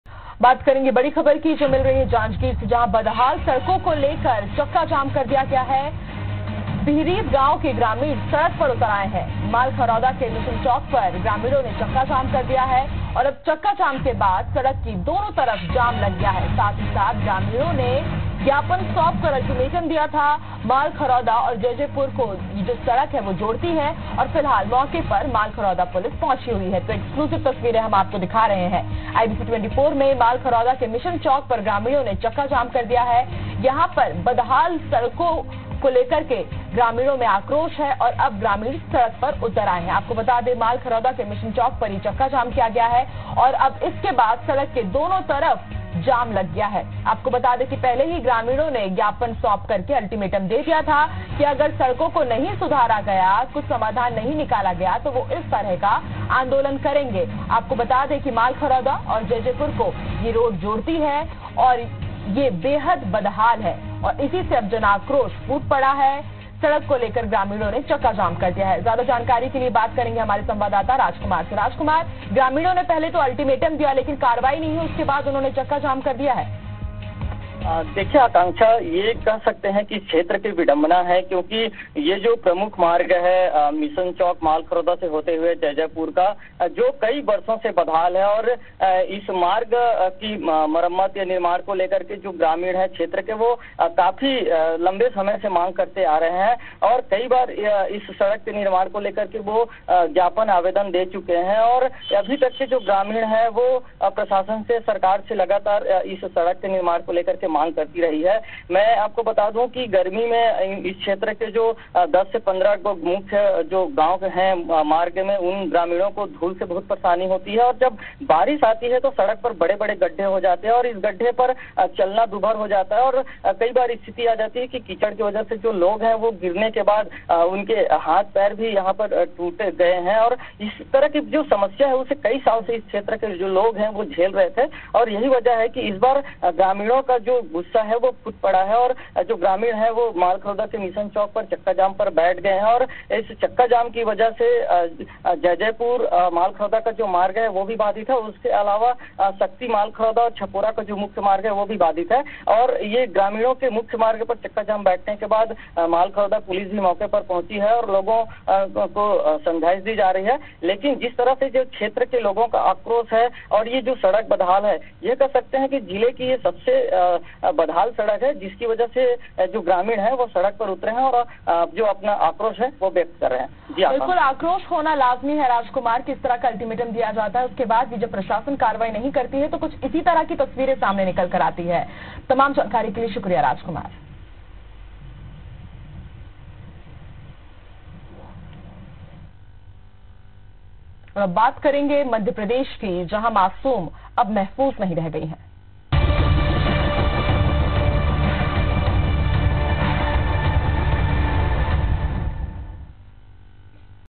जांजगीर. बदहाल सड़क के निर्माण को लेकर ग्रामीणों ने मालखरौदा के मिशन चौक में किया चक्काजाम.
janjgir roadproblem roadblock malkharoda-jaijaipurroad